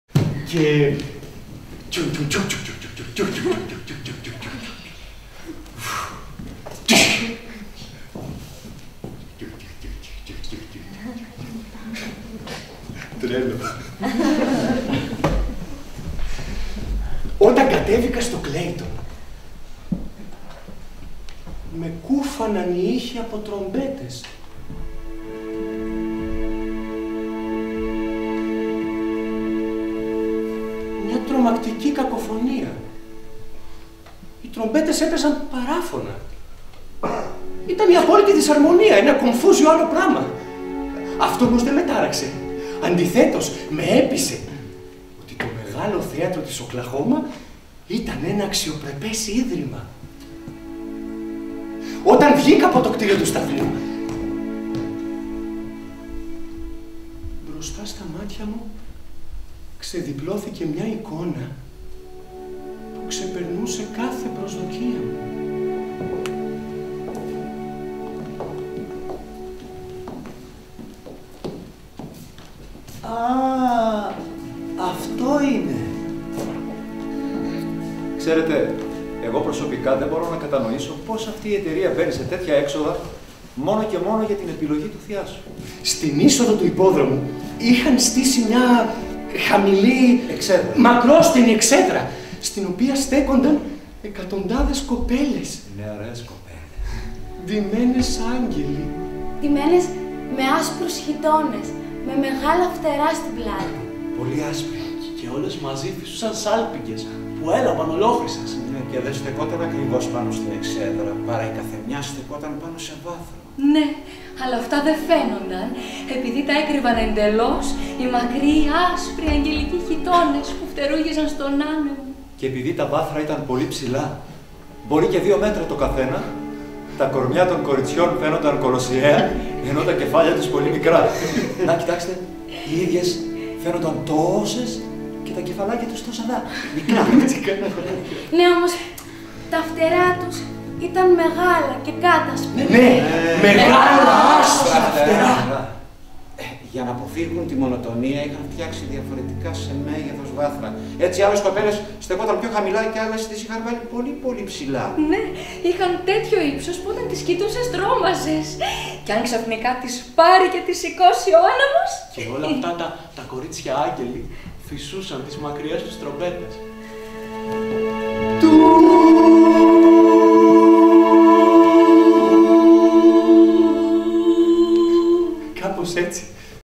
ΚΘΒΕ 2011: Déjà Vu (2/8)
Μια κωμική ιστορία εμπνευσμένη από μοτίβα έργων του Φραντς Κάφκα.

Συγγραφέας: Φραντς Κάφκα
Διασκευή - σκηνοθεσία - φωτισμοί: Τσέζαρις Γκραουζίνις
Μετάφραση: Αλεξάνδρα Ιωαννίδου
Σκηνικά - κοστούμια: Βιτάουτας Νάρμπουτας
Μουσική: Μαρτίνας Μπιαλομπζέσκις
Βοηθός σκηνογράφου: Κλεοπάτρα Αλαγιαλόγλου
Οργάνωση παραγωγής: Χριστίνα Ζαχαροπούλου

Ηθοποιοί
Γιάννης Καραούλης
Γιώργος Καύκας
Μαργαρίτα Παπαγιάννη
Γιώργος Σφυρίδης
Ευανθία Σωφρονίδου
Κωνσταντίνος Χατζησάββας